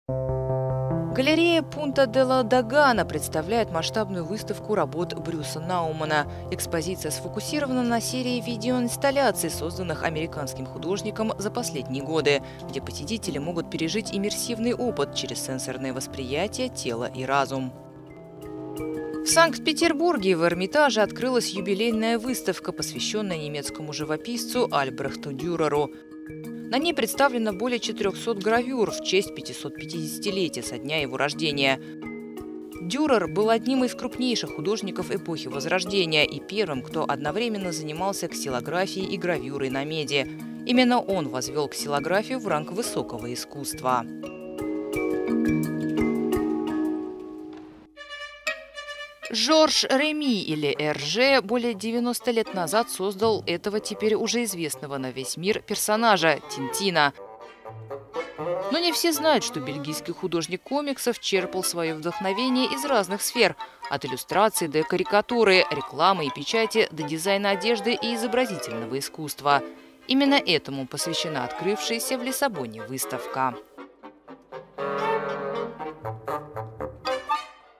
Rendez-vous: от гравюр Дюрера до Тинтина
Самые заметные выставки Европы: работы Брюса Наумана, гравюры Дюрера и комиксы Эрже